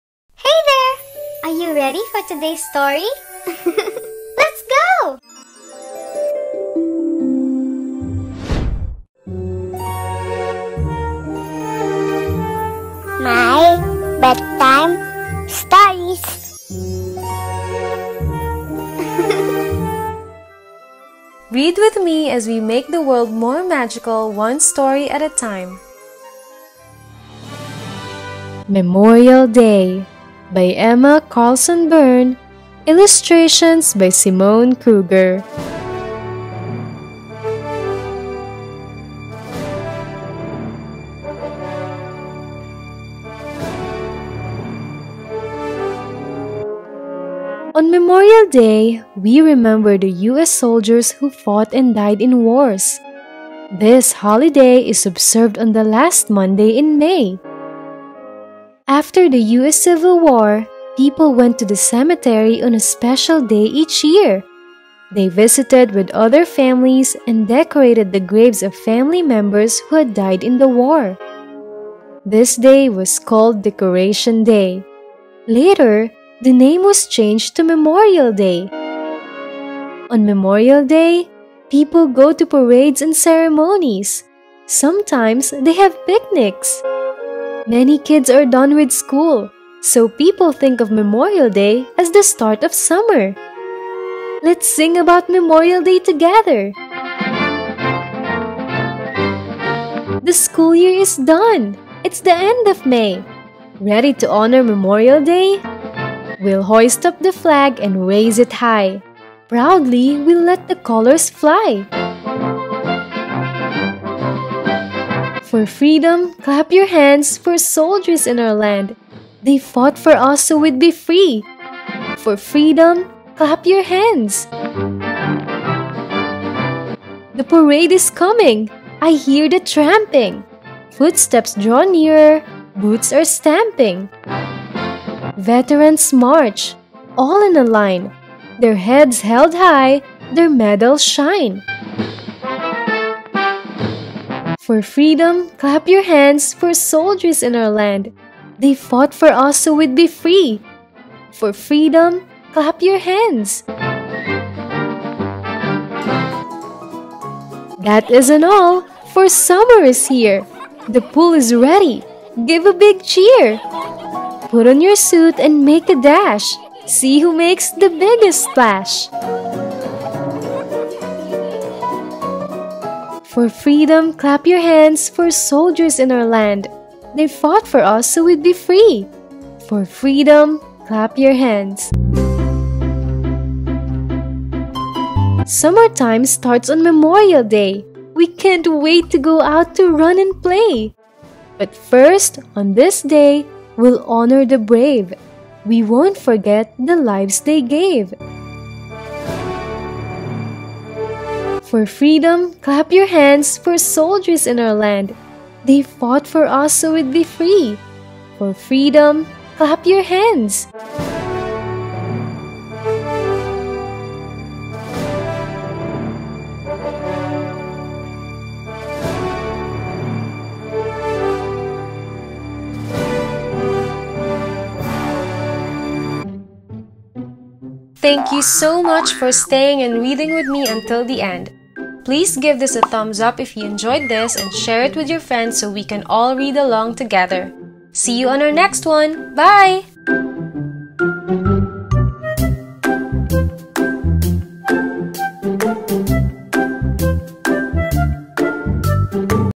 Memorial Day by Emma Carlson Berne | Honoring Soldiers' Sacrifices | Memorial Day Read Aloud
"Memorial Day honors soldiers who fought and died for the United States. On this day, communities gather to remember soldiers’ sacrifices and celebrate the beginning of summer. "

You can purchase this book on Amazon!!! Here's the link: